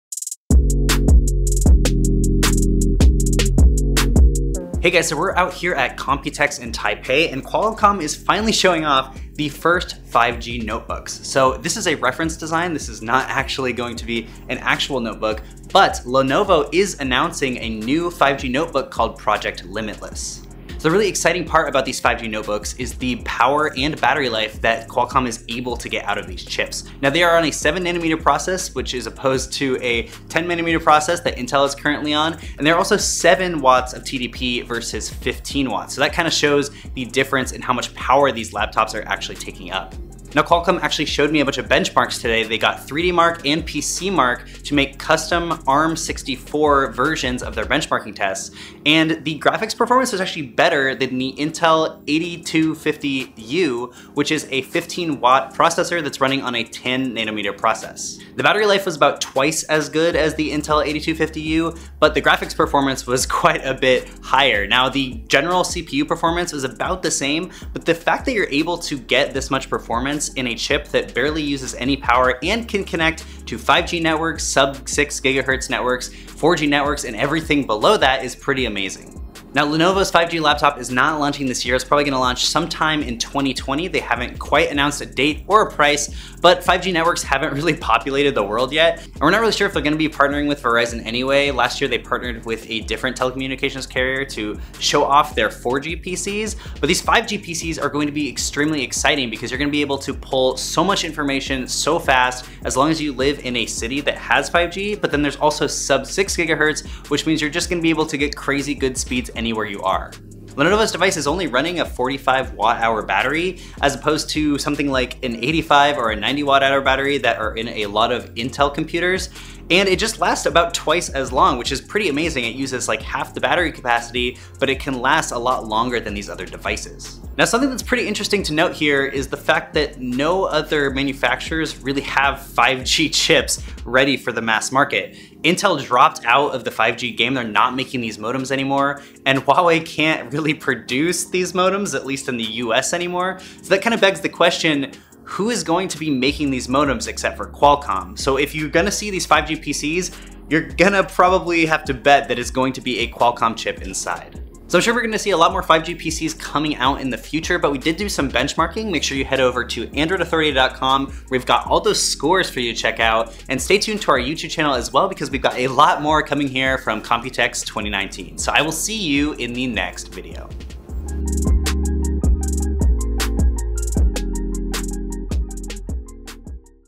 Qualcomm 8cx benchmarked: Faster than Intel's i5 8250U?
We finally got our hands on Qualcomm's new 8cx SOC. Is it faster than Intel's comparable chip?